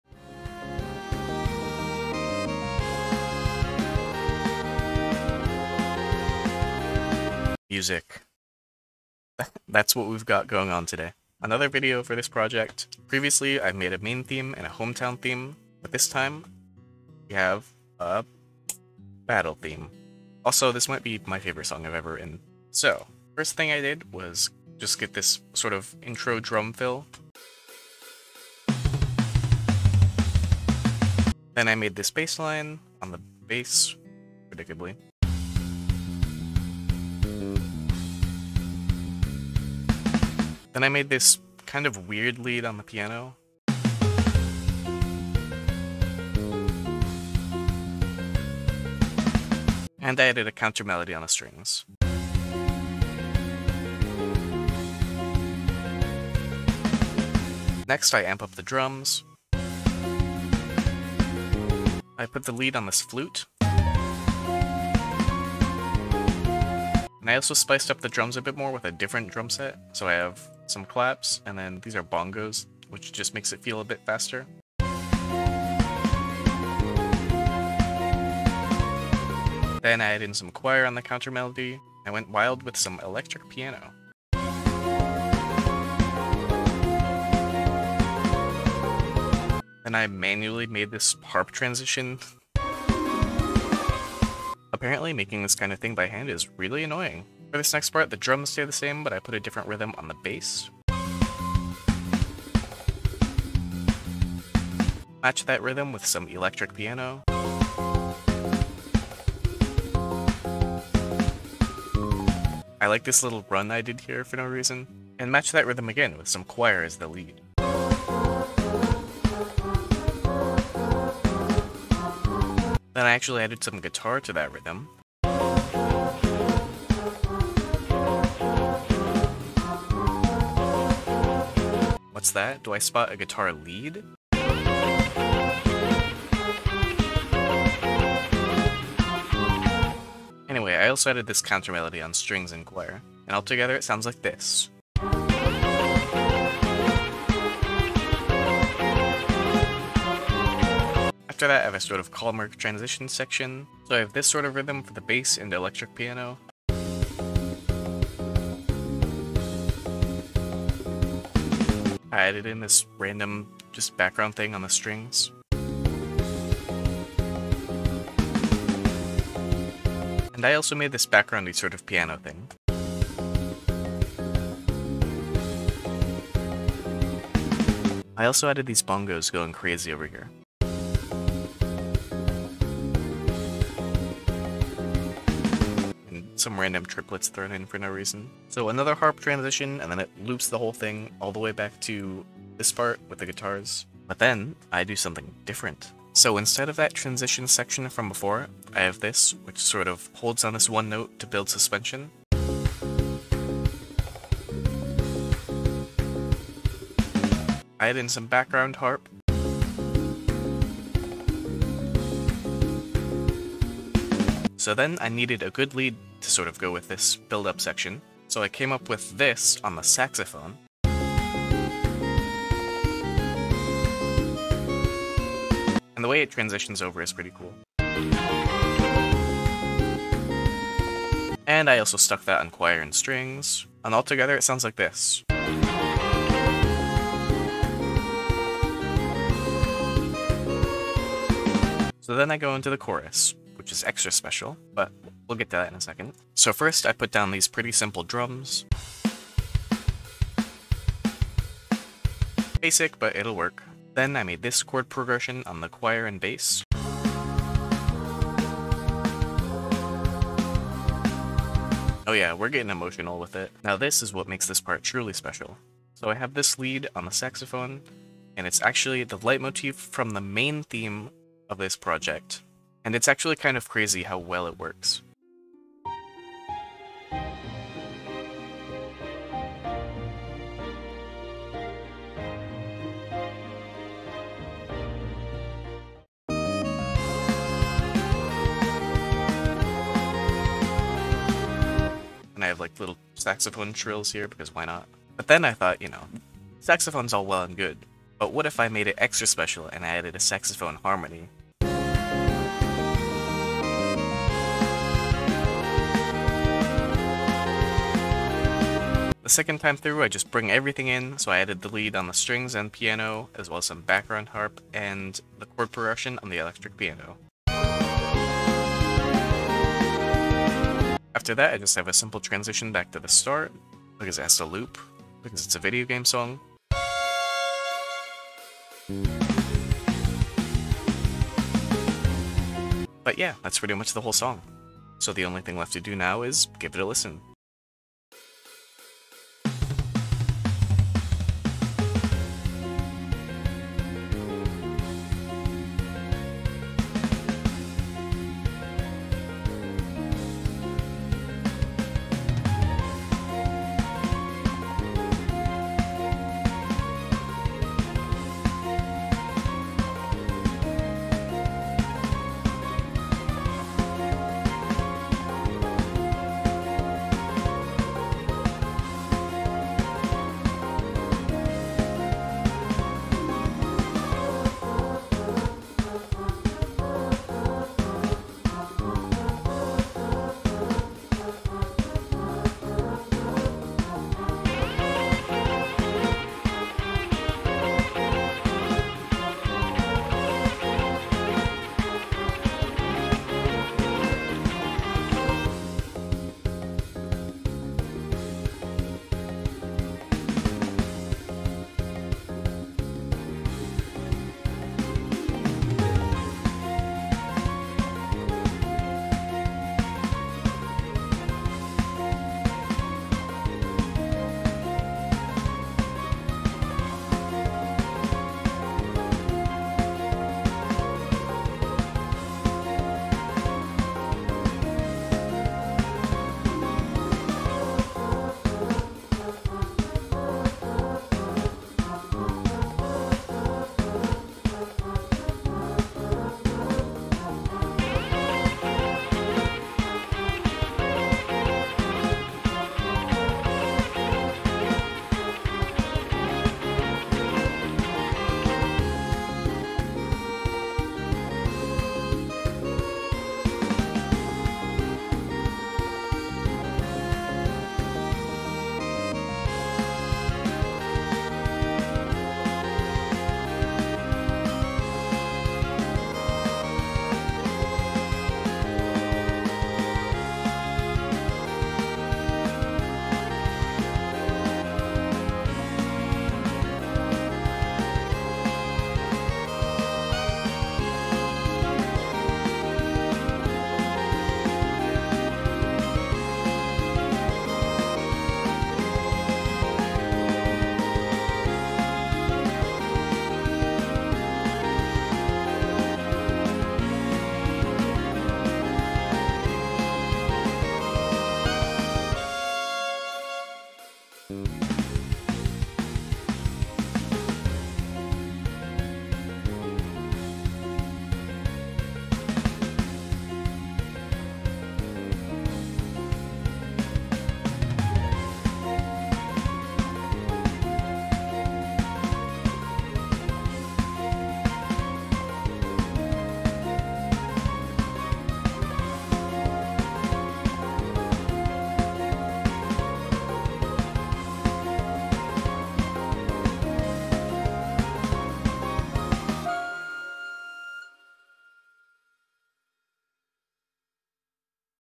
I Made a JRPG Battle Theme
Skip to 5:39 for the full song.

Fun fact: this was actually the second song I wrote for this project, but I wanted to release these in a somewhat coherent order, so I delayed it until now. This turned out to be one of my favourite songs I've ever written, so I hope you all like it too! I'll probably make more battle songs for this project, by the way.
I was so close to naming this song "In Contention With Intention." You guys are so lucky that I changed my mind.